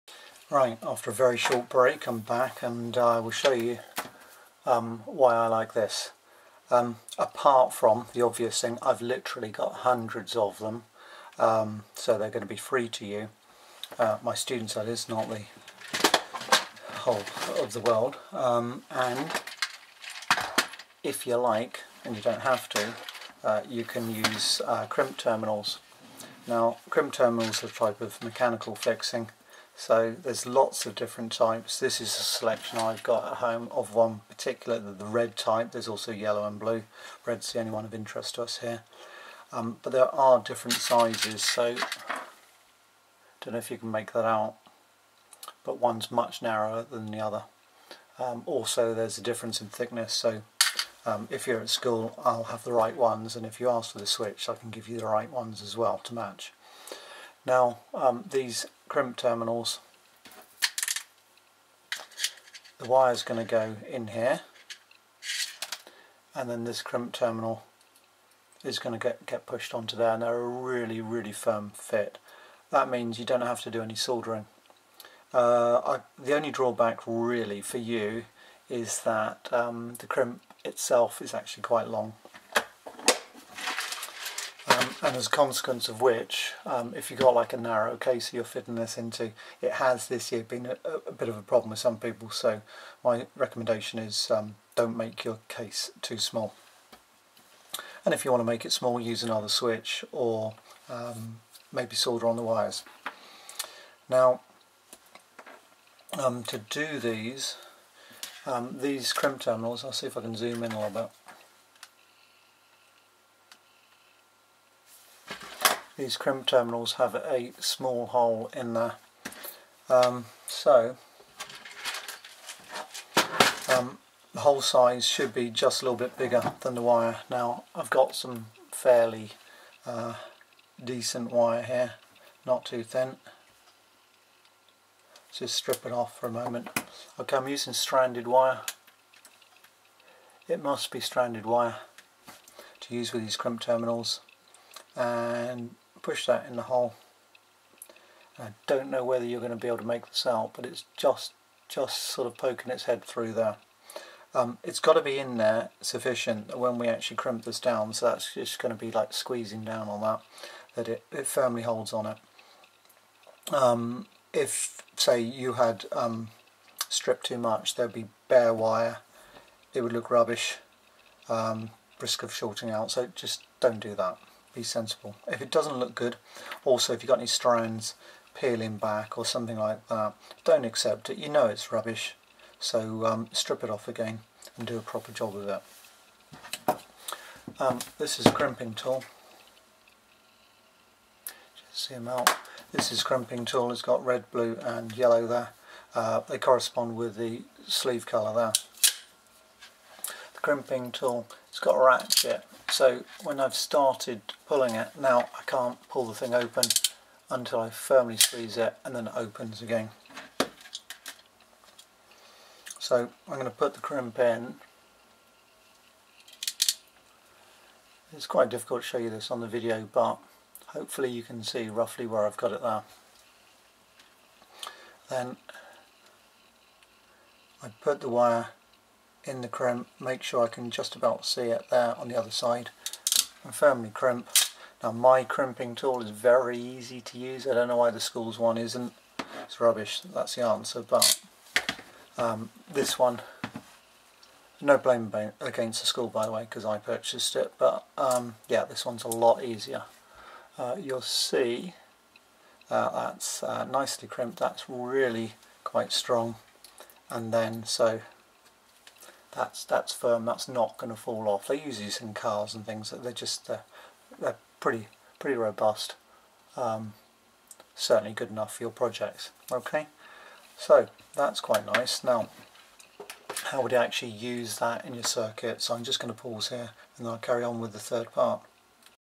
Adding crimp terminals to DPST rocker switch
In this video I show you how to add crimp terminals to a DPST rocker switch. We have a lot of these, so if you are in my class then you should be using them.